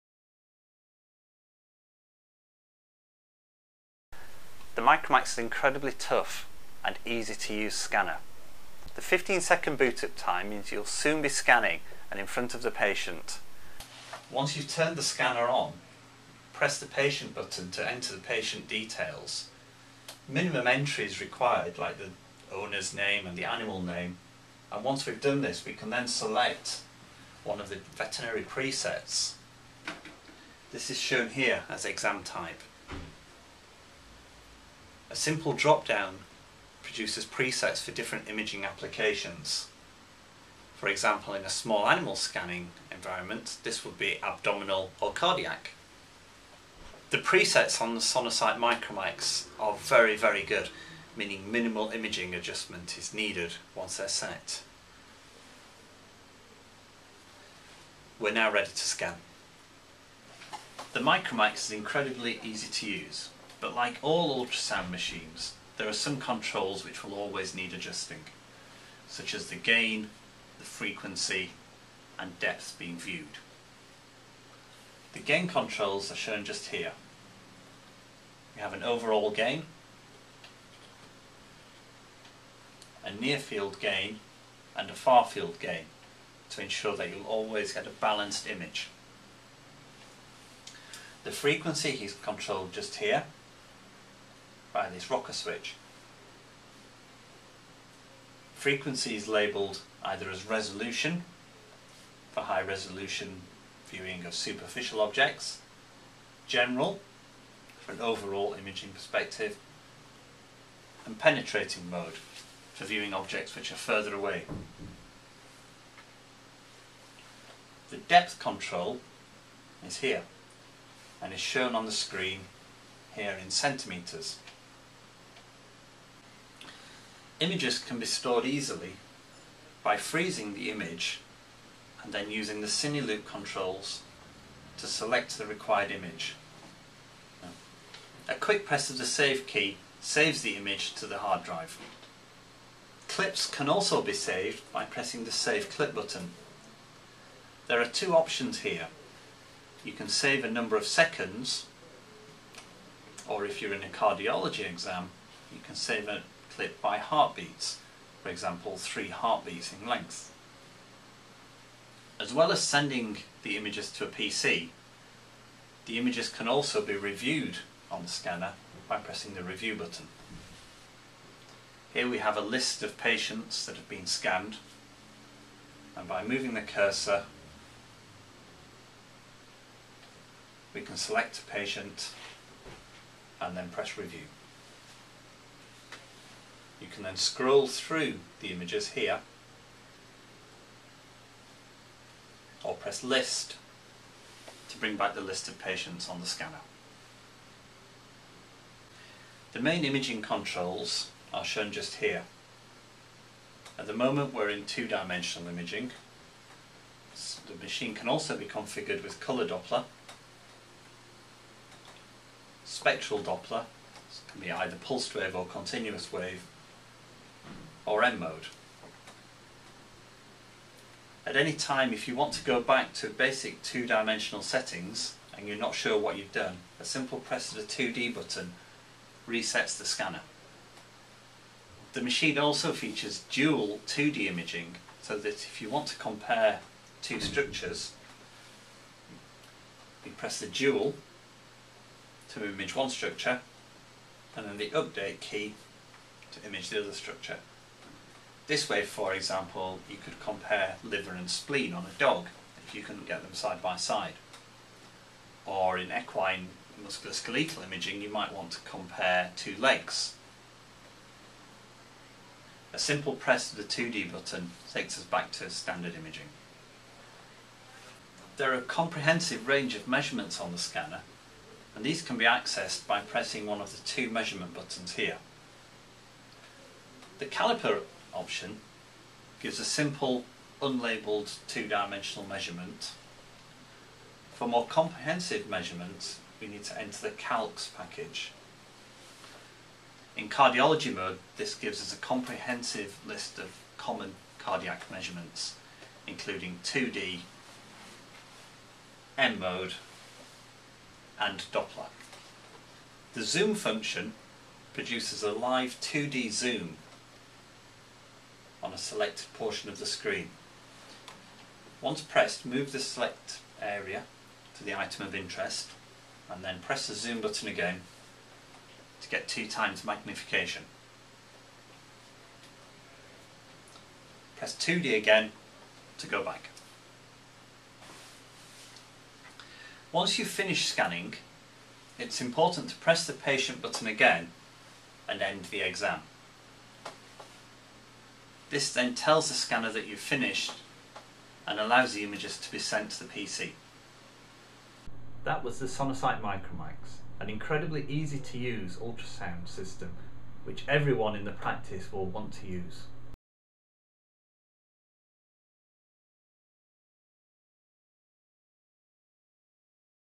BCF Technology, Sonosite MicroMaxx veterinary ultrasound scanner -- Step by step instructional guide
Nigel Perry, Veterinary Ultrasound Manager at BCF Technology, takes you through a simple step by step guide to the controls of the portable Sonosite MicroMaxx veterinary ultrasound machine.  This scanner has been specifically designed to be easy to operate, with a quick start up time, meaning that vets in your small animal or equine practice will use this equipment more.